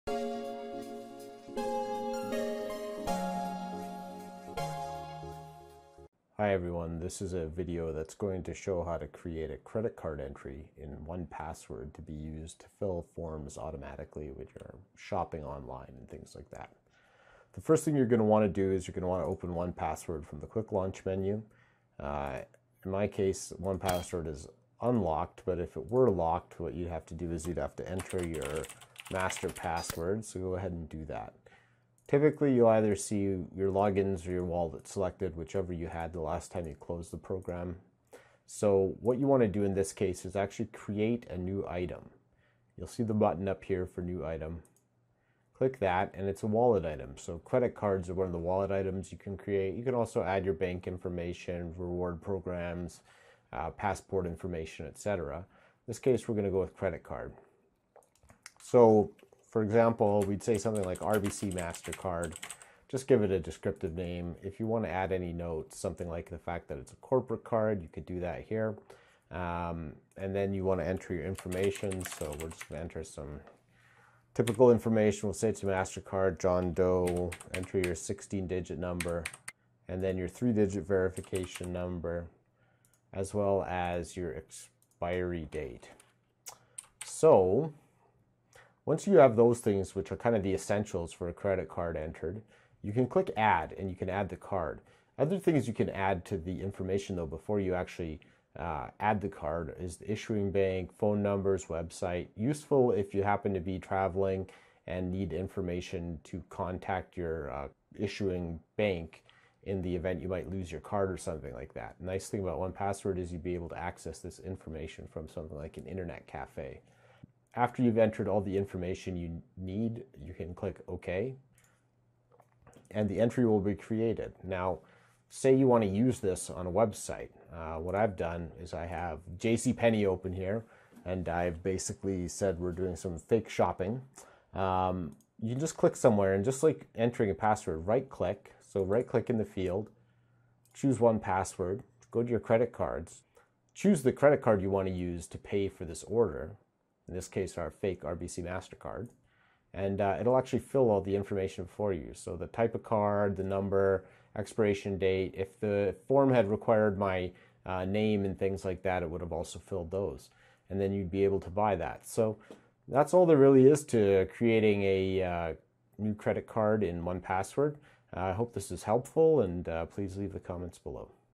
1Password - How to add a credit card and use it
Short video illustrating how to add a credit card to the 1Password app and use that credit card for online purchases.